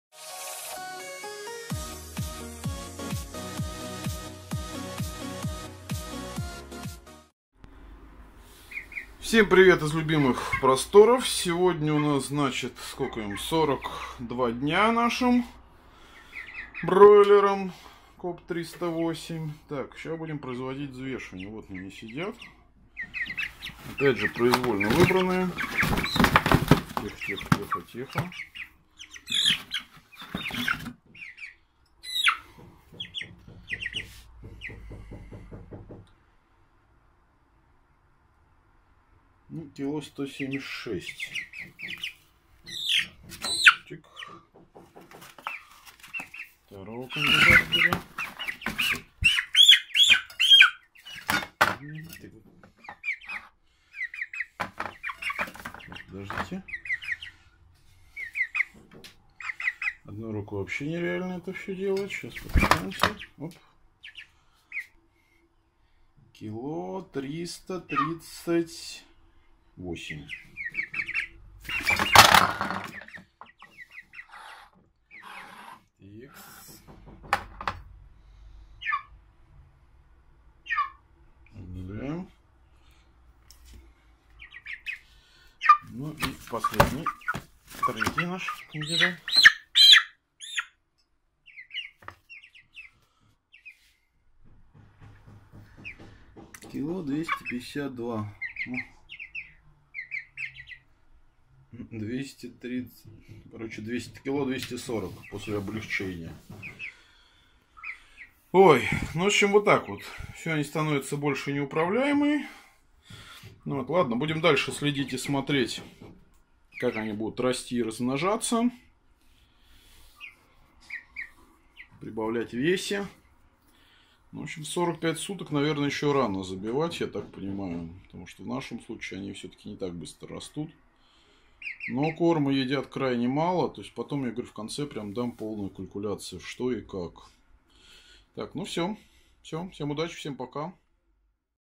Бройлеры 42 дня
Продолжаем наш эксперимент. Куриное мясо без ГМО, лекарств,  усилителей роста и вкуса. Только свежая вода и корм.